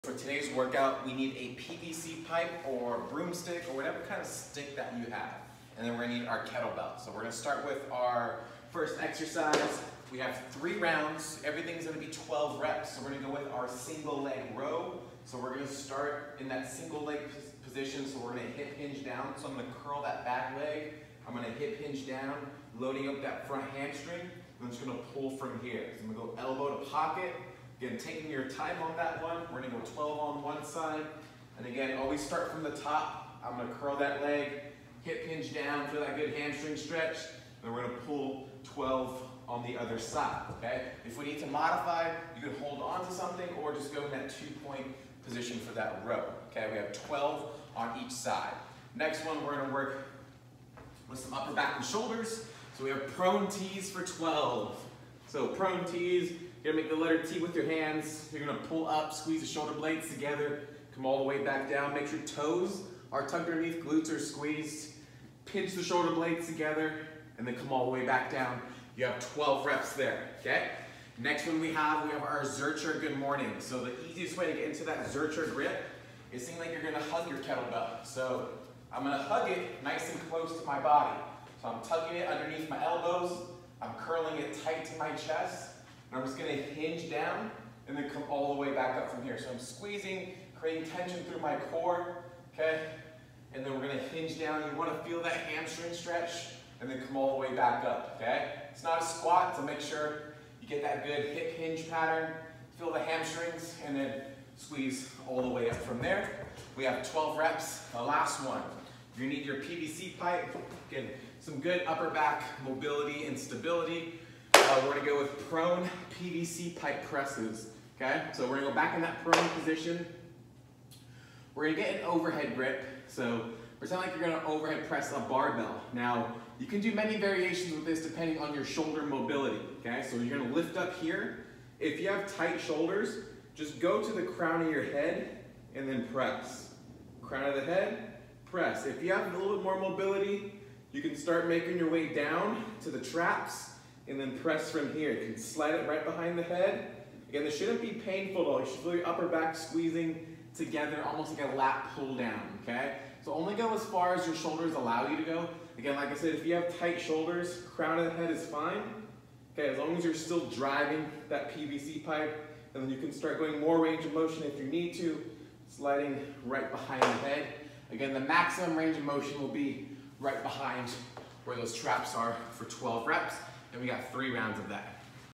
4-22-20 At Home KB/DB Workout Part 1
Part 1
Posterior Chain Warm up
3 rounds
12 ea. Single Leg Rows
12 Prone Ts 12
12 KB Zercher Good Mornings
12 Prone PVC Pipe Press